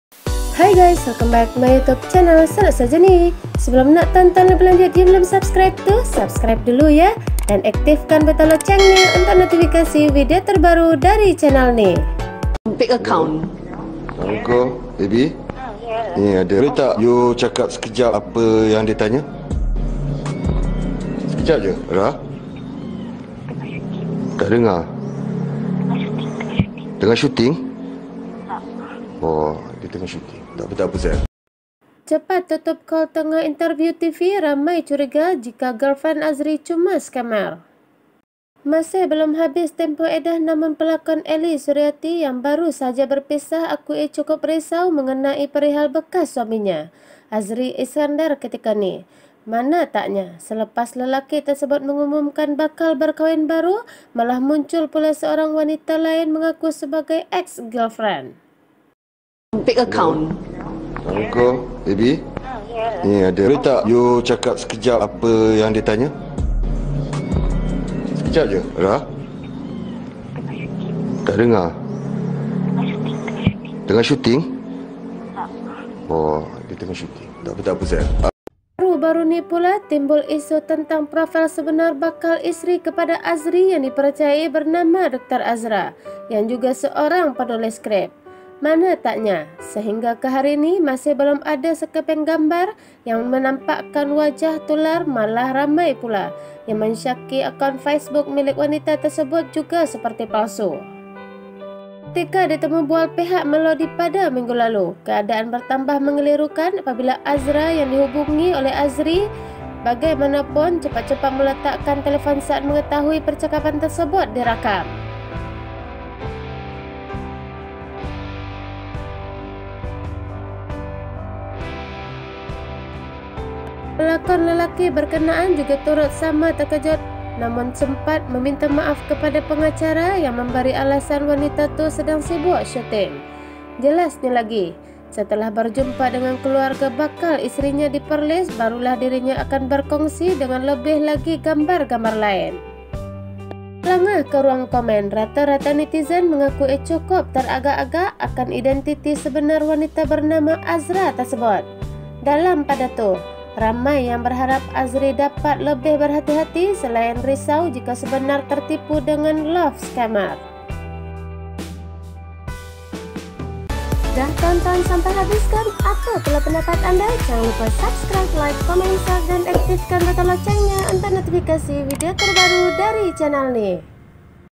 Cepat Tutup Call Tengah Interview TV, Ramai Curiga Jika GF Azri Iskandar Cuma Scammer
Sumber : Oh My Media